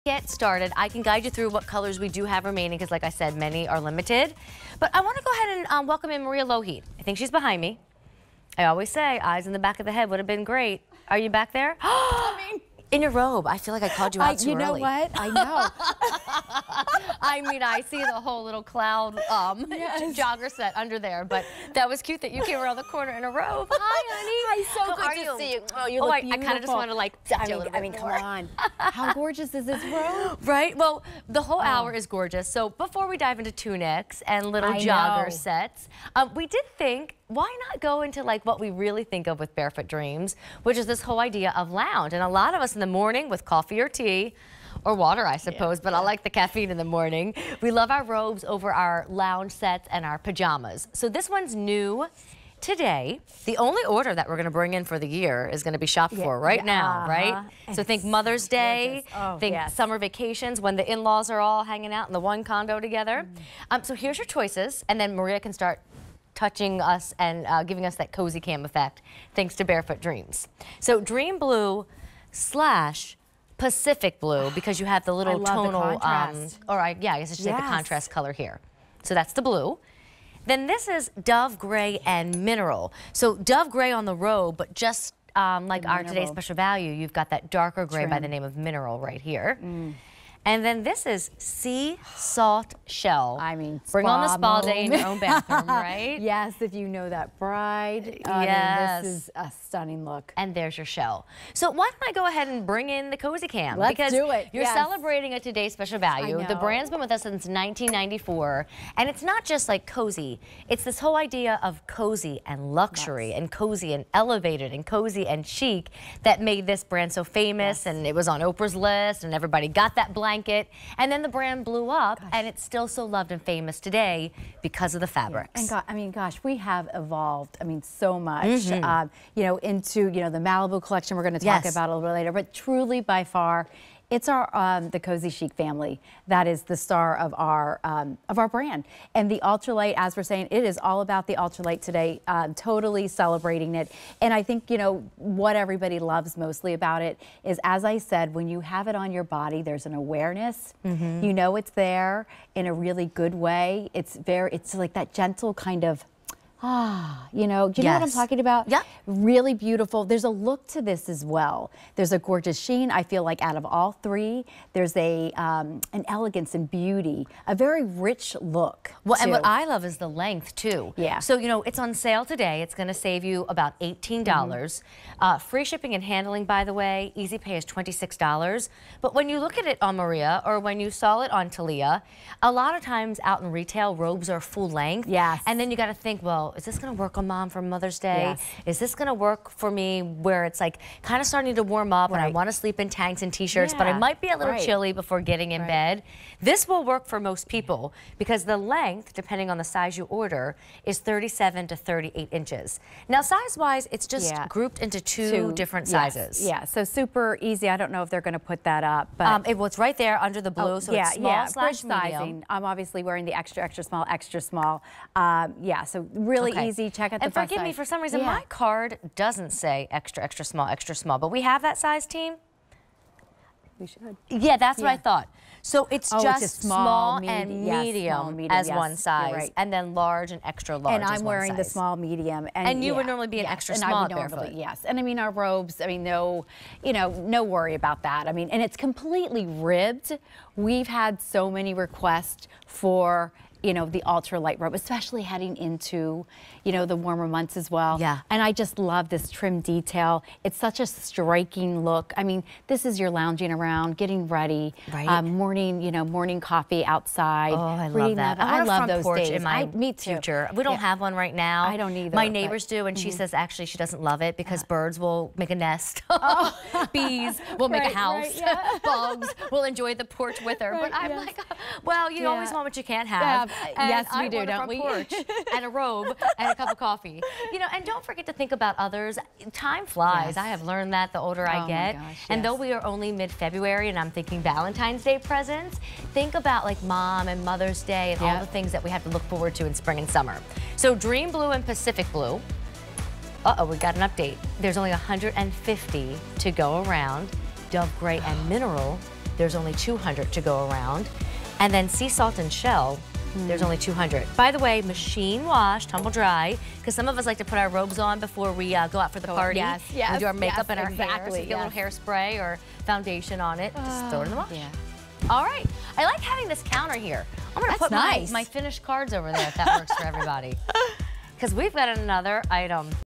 Barefoot Dreams CozyChic Ultra Lite Ribbed Short Robe on QVC
Barefoot Dreams CozyChic Ultra Lite Ribbed Short Robe

Grab a stack of your favorite reads, a steamy mug, and spend the day (or the week) in this soft and sumptuous  CozyChic Ultra Lite robe with allover ribbing. From Barefoot Dreams.